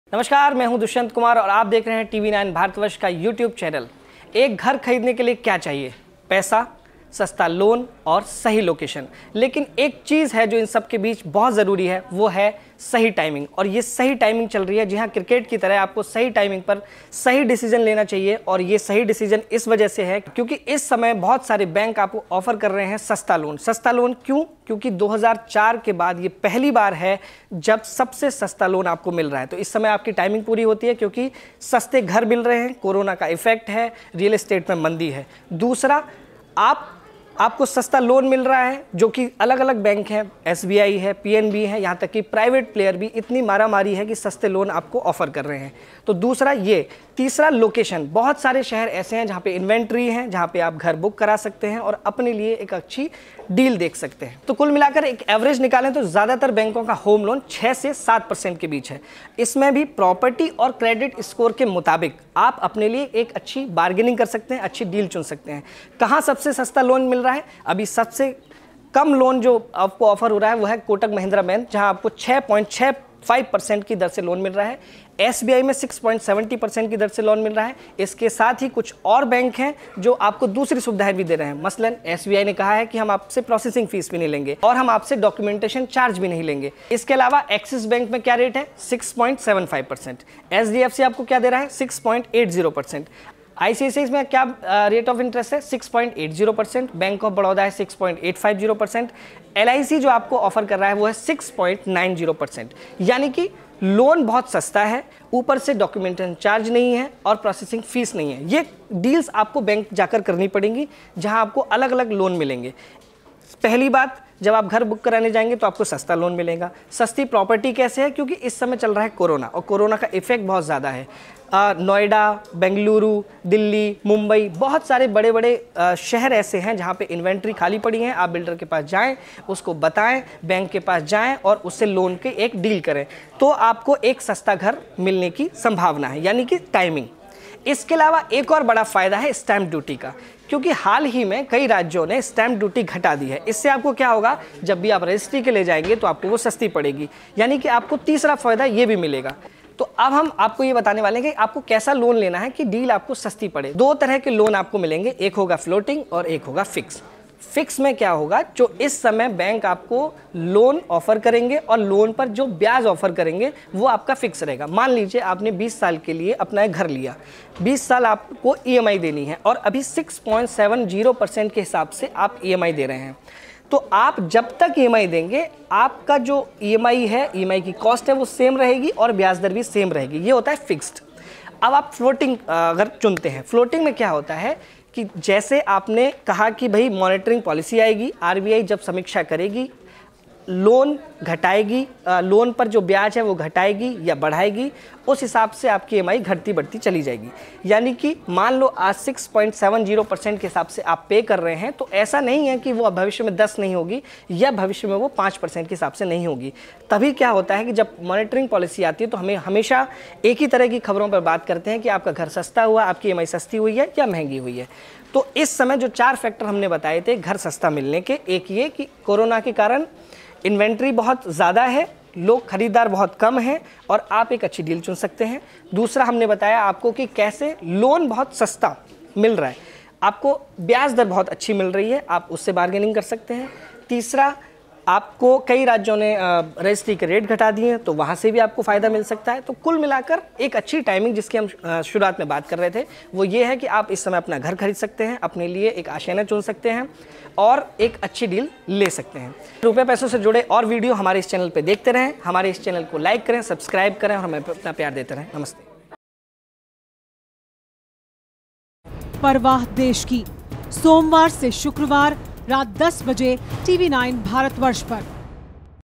सस्ता घर लेना है तो ये बेस्ट टाइम है ! | Home Loan
अपना घर लेना हर इंसान की जिंदगी का एक ख्वाब होता है. लेकिन होम लोन की ऊंची दरें अक्सर इस ख्वाब पर भारी पड़ जाती हैं. लेकिन अब आपका सपना पूरा हो सकता है. दरअसल लॉकडाउन के बाद सरकारी और प्राइवेट बैंक काफी कम दरों पर होम लोन मुहैया करा रहे हैं. घर लेते समय सस्ती होम लोन दरों के अलावा भी कई चीजों का काफी ख्याल रखना पड़ता है, तो आइए जानते हैं कि आप कैसे इन बातों का ध्यान रखते हुए आप घर लेते हुए अपना बंपर फायदा कर सकते हैं?

Credit: Banking | Samarth/Producer | Ritu/Editor